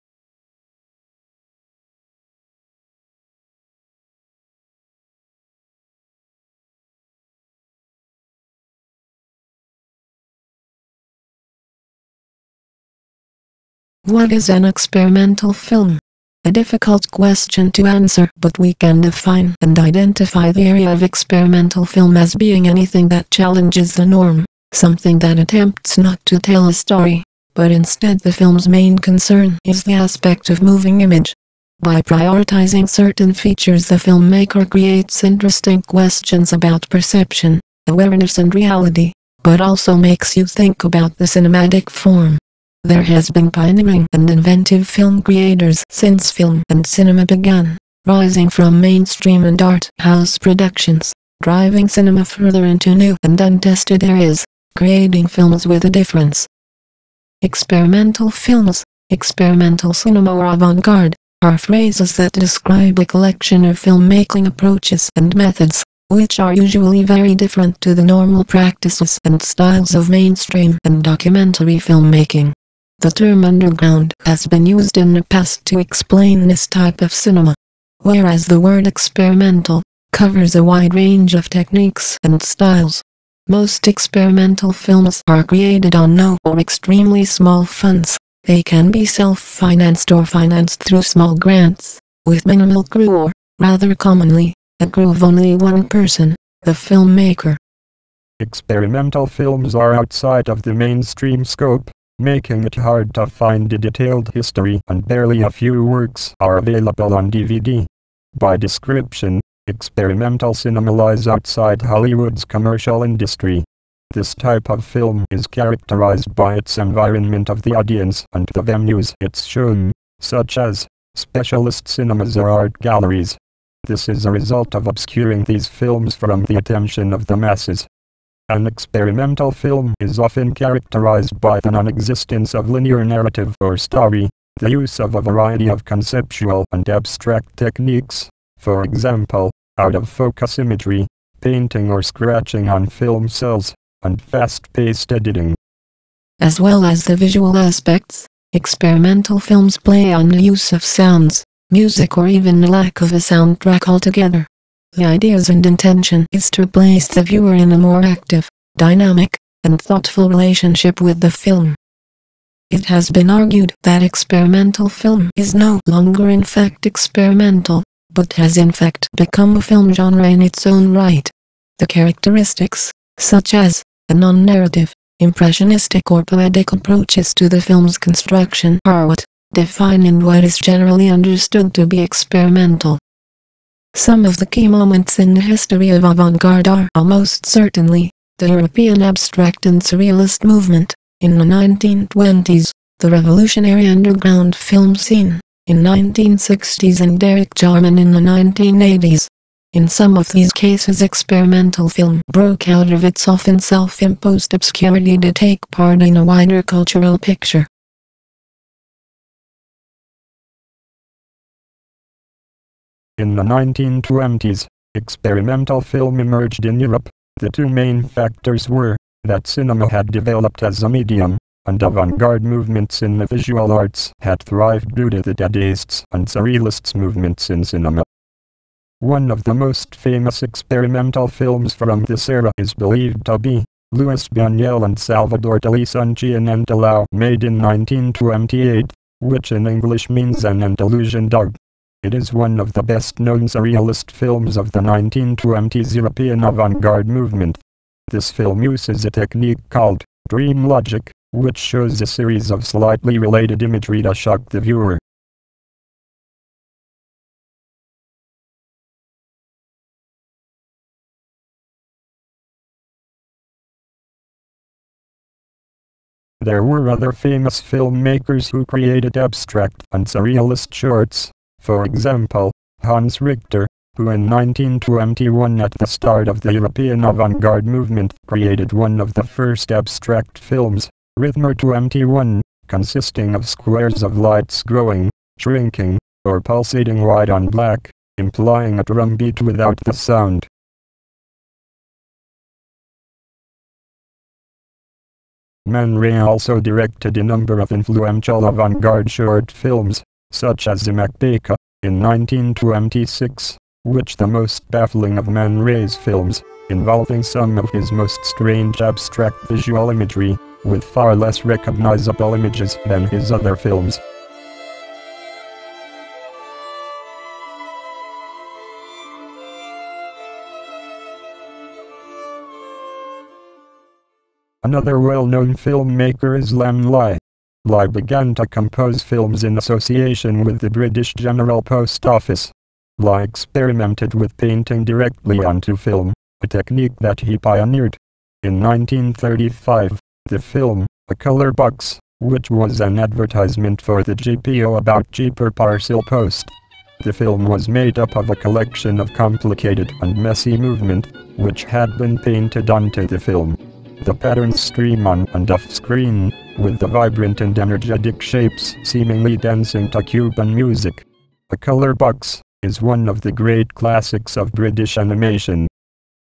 Avant Garde - Experimental Film (Part 1)
This was the main part of my 3rd year project - an experimental documentary about the history of avant garde film and filmmakers. (Part One)